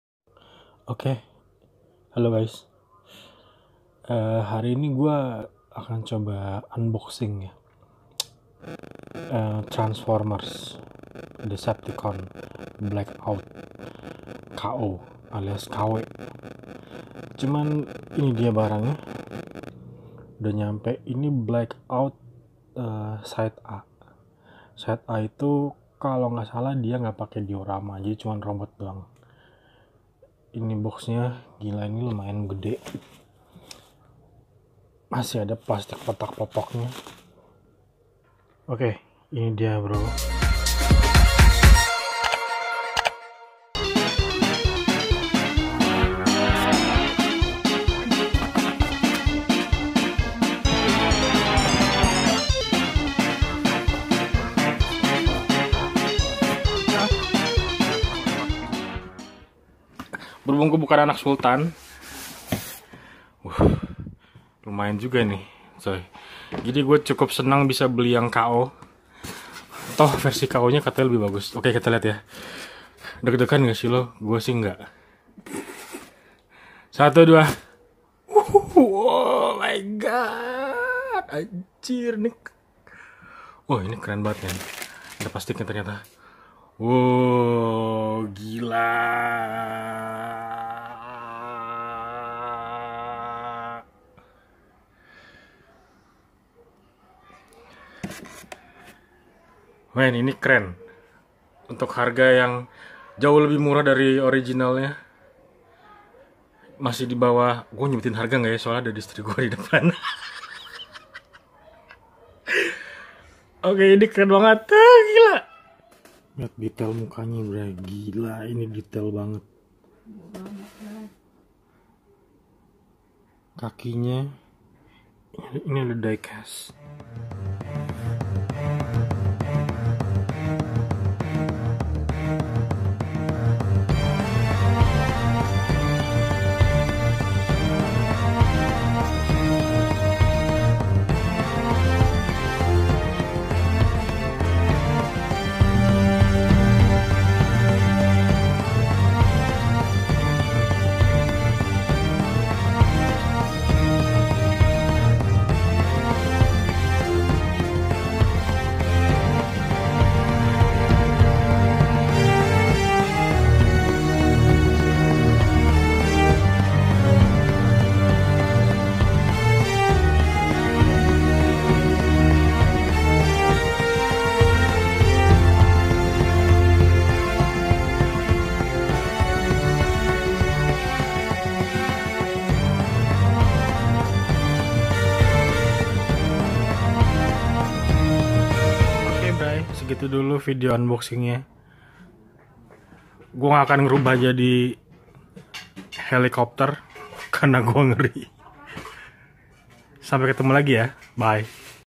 Wei Jiang HIDE SHADOW SIDE A (KO Upscale Studio Series Blackout: Yamers Transformers Unboxing Review
Yamers...

Selain makan mie gue juga suka toys, dan barang barang cowok lainnya.

Nah yang ini unboxing Transformers KO dari weijiang yang selama ini gue cuma nonton review review dari orang dan gue jadi penasaraaaaaann banget...

Dan akhirnyaaa... LUAARRR BIASAAAAAAHHHH.. MURAAAH DAN KEREEEN ABISSSSS

Cekidot Guys